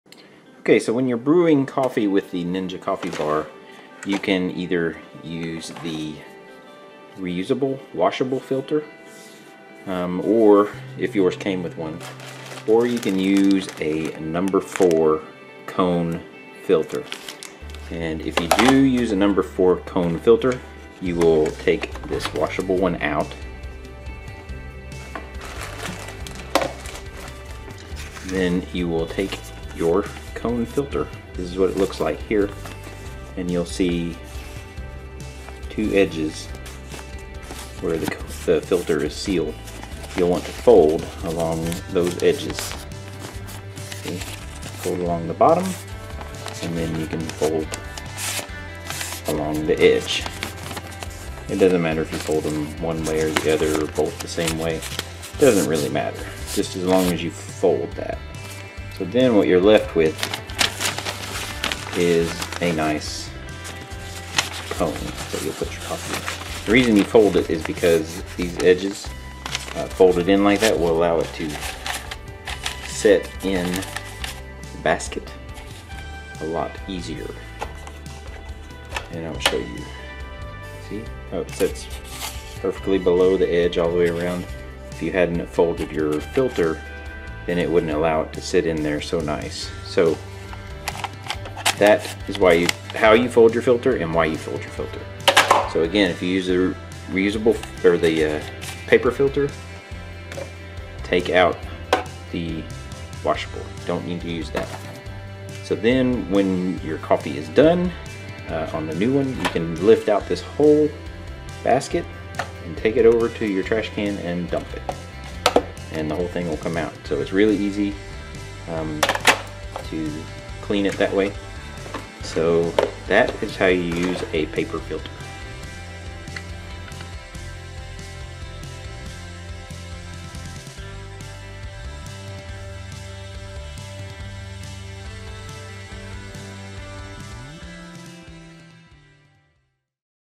Using Paper Filters - Ninja Coffee Bar
Here is a quick tutorial on how to fold and use paper filters in your New Ninja Coffee Bar.

Get your new Ninja Here!
New Hot Cold Brew System